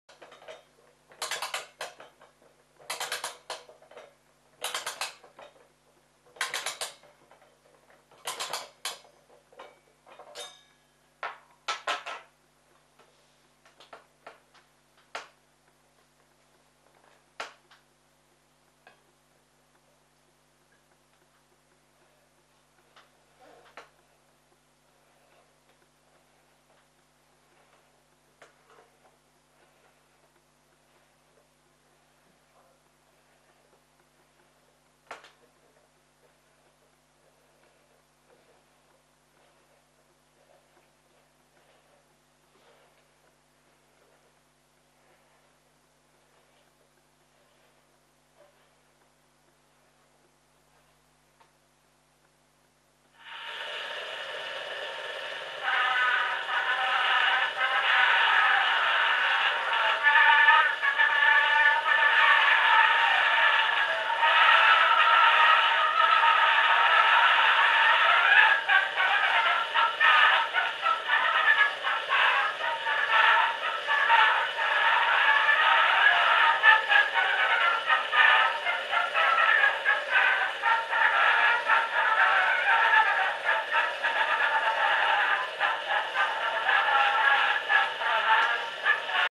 kiddyphone
From my collection, a nice little antique tin german made grammophone by bing...